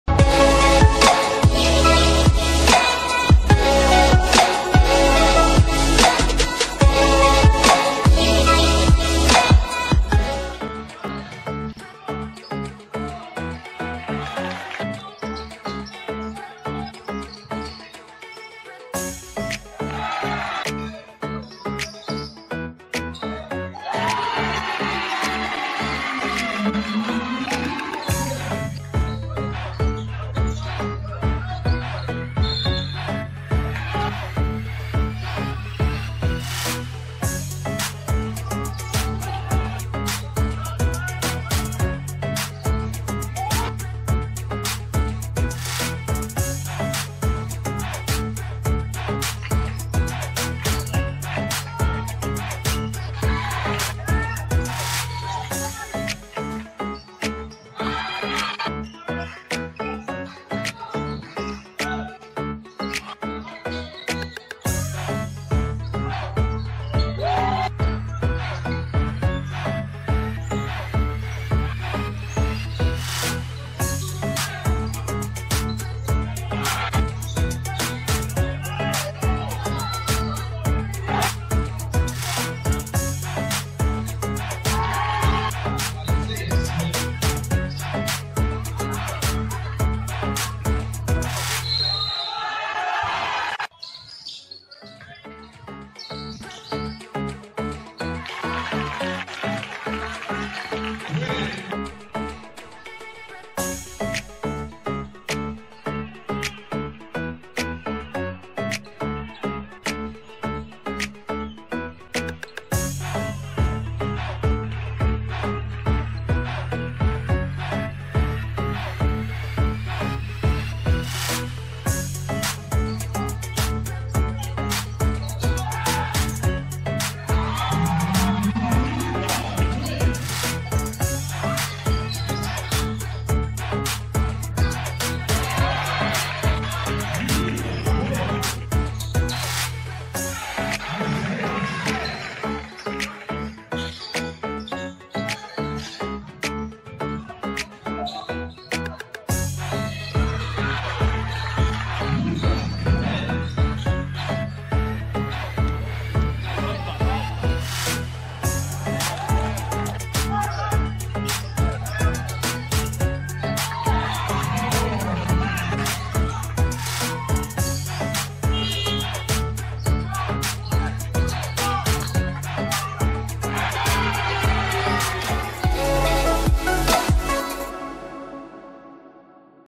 Luca Quinn basketball highlights -- ScoutU
Luca Quinn is a 2025 guard from Olean H.S. in New York.